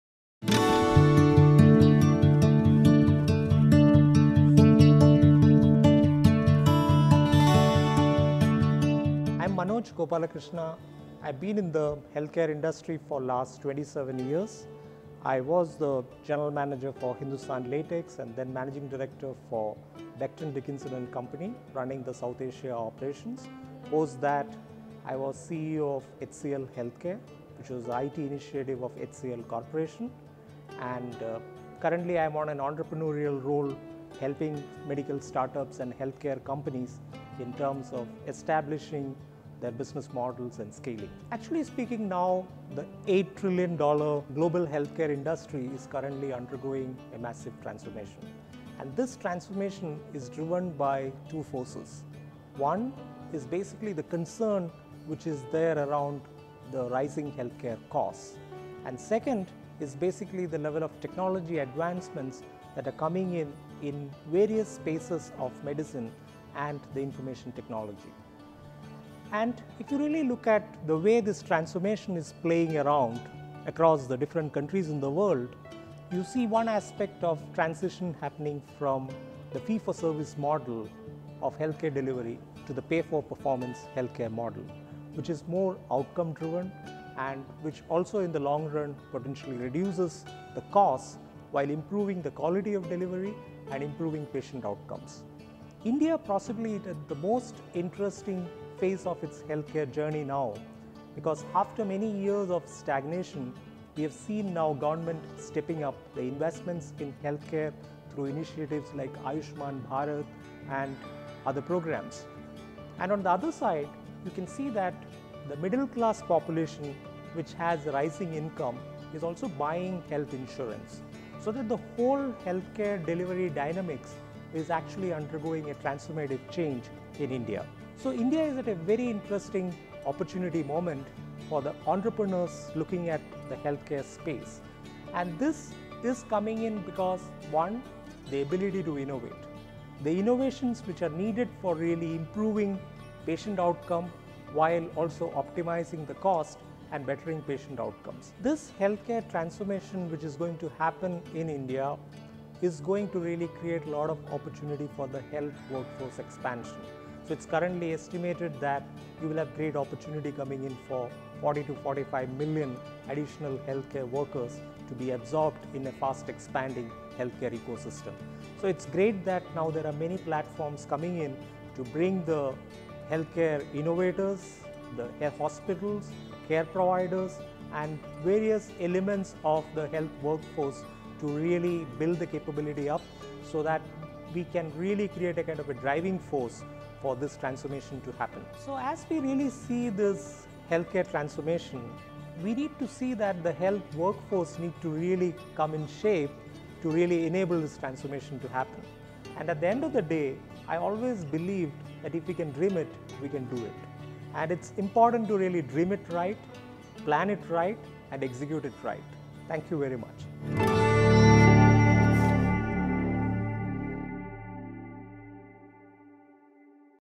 Mr. Manoj Gopalakrishna
Rxcruit is pleased to share the leadership insights by Mr. Manoj Gopalakrishna. He rightly says, “if u can dream it you can do it”. Let’s all have a dream and achieve it.